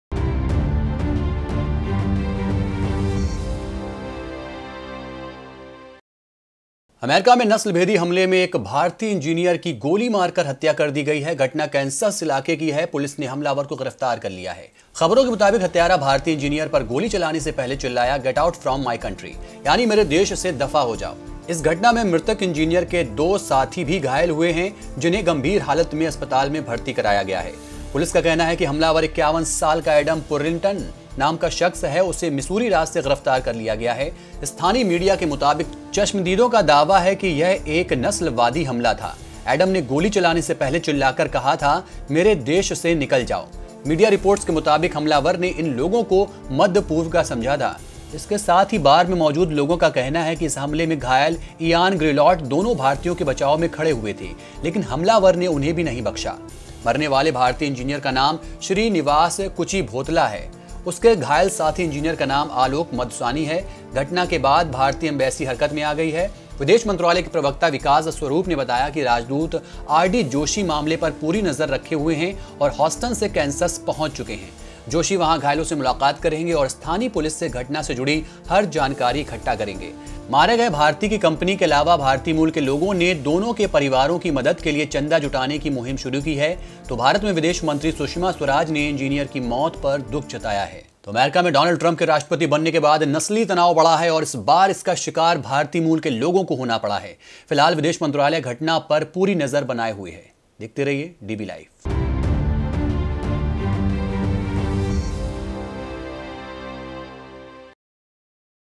DB LIVE | 24 FEB 2017 | US hate attack: Indian engineer shot dead, 2 critically injured in Kansas
अमेरिका में नस्लभेदी हमले में एक भारतीय इंजीनियर की गोली मारकर हत्या कर दी गई। घटना कैनसस इलाके की है। पुलिस ने हमलावर को गिरफ्तार कर लिया है। खबरों के मुताबिक, हत्यारा भारतीय इंजिनियर पर गोली चलाने से पहले चिल्लाया get out from my country यानी मेरे देश से दफा हो जाओ। ' इस घटना में मृतक इंजीनियर के दो साथी भी घायल हो गए, जिसे गंभीर हालत में अस्पताल में भर्ती कराया गया है। पुलिस का कहना है कि हमलावर 51 साल का एडम पुरिंटन नाम का शख्स है। उसे मिसूरी राज्य से गिरफ्तार कर लिया गया है। स्थानीय मीडिया के मुताबिक चश्मदीदों का दावा है कि ये एक नस्लवादी हमला था। एडम ने गोली चलाने से पहले चिल्लाकर कहा था, 'मेरे देश से निकल जाओ।'  मीडिया रिपोर्ट्स के मुताबिक हमलावर ने इन लोगों को मध्य-पूर्व का समझ लिया था। इसके साथ ही बार में मौजूद लोगों का कहना है कि इस हमले में घायल इएन ग्रिलॉट दोनों भारतीयों के बचाव में खड़े हुए थे। लेकिन हमलावर ने उन्हें भी नहीं बख्शा। मरने वाले भारतीय इंजिनियर का नाम श्रीनिवास कुचिभोतला है। उसके घायल साथी इंजिनियर का नाम आलोक मदासनी है। घटना के बाद भारतीय एंबेसी हरकत में आई गई है। विदेश मंत्रालय के प्रवक्ता विकास स्वरुप ने बताया कि राजदूत आरडी जोशी मामले पर पूरी नजर बनाए हुए हैं और हॉस्टन से कैनसस पहुंच चुके हैं। जोशी वहां घायलों से मुलाकात करेंगे और स्थानीय पुलिस से घटना से जुड़ी हर जानकारी इकट्ठा करेंगे। गारमिन कंपनी के अलावा भारतीय मूल के लोगों ने दोनों के परिवारों की मदद के लिए चंदा जुटाने की मुहिम शुरू की है। तो भारत में विदेश मंत्री सुषमा स्वराज ने इंजीनियर की मौत पर दुख जताया है। अमेरिका में डोनाल्ड ट्रंप के राष्ट्रपति बनने के बाद नस्लीय तनाव बढ़ा है और इस बार इसका शिकार भारतीय मूल के लोगों को होना पड़ा है। फिलहाल विदेश मंत्रालय घटना पर पूरी नजर बनाए हुए हैं।